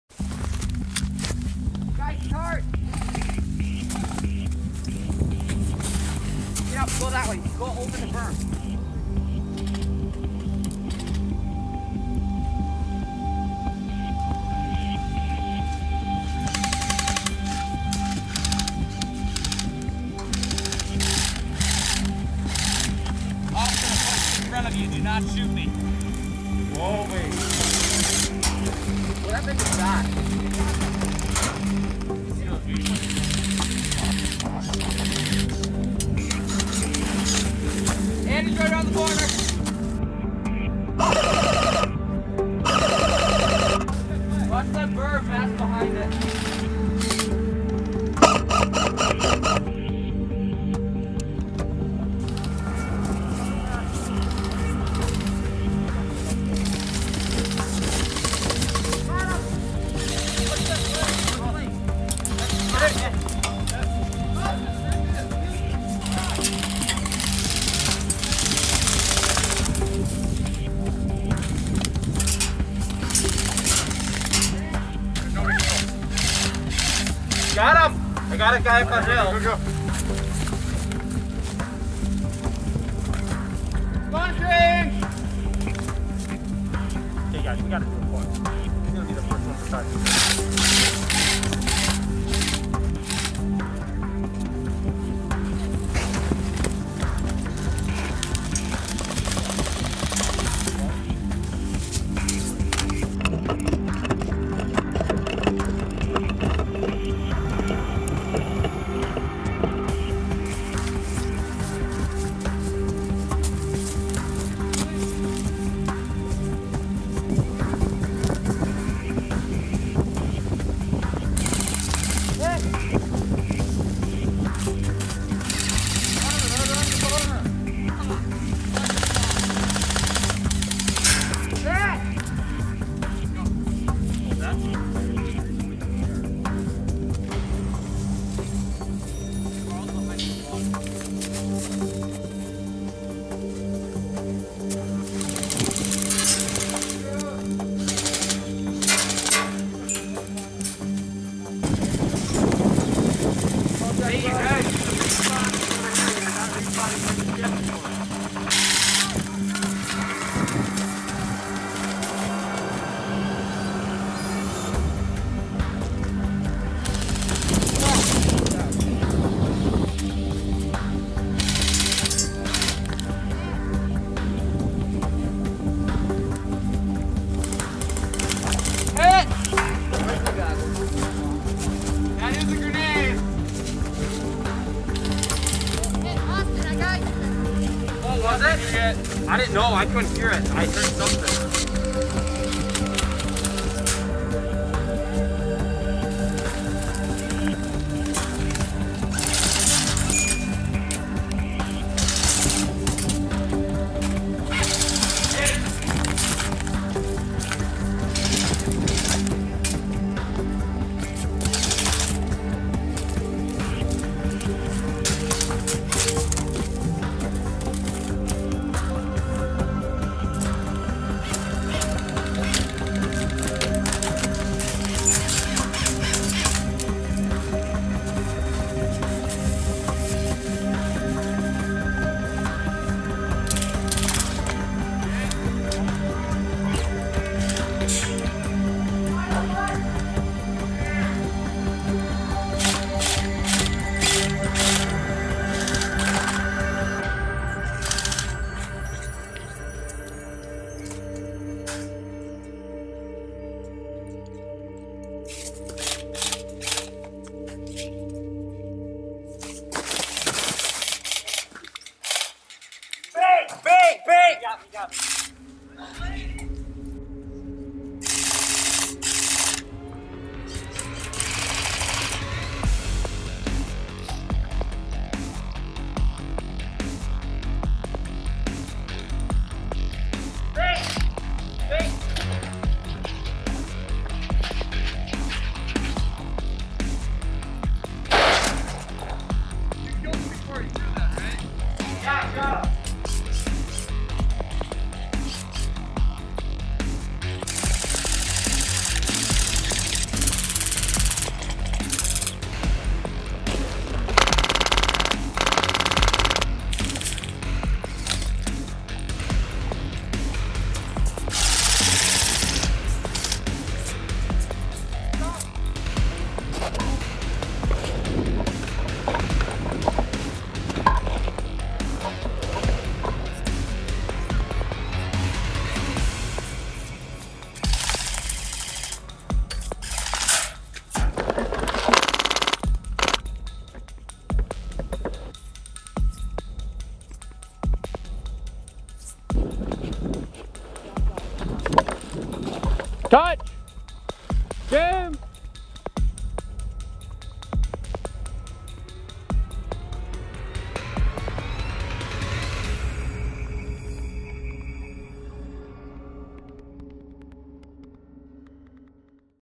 The Scrapyard: Capture and Defend
Game of Capture and Defend at the Scrapyard! Defending team must defend several checkpoints around the field. Attacking team starts at their initial spawn and must capture all of the checkpoints in order. Once a checkpoint is captured, defending team's spawn point gets pushed back to a new location behind the second checkpoint. Attacking team's spawn moves up to the recently captured checkpoint. Game is over when all checkpoints are captured. Infinite lives.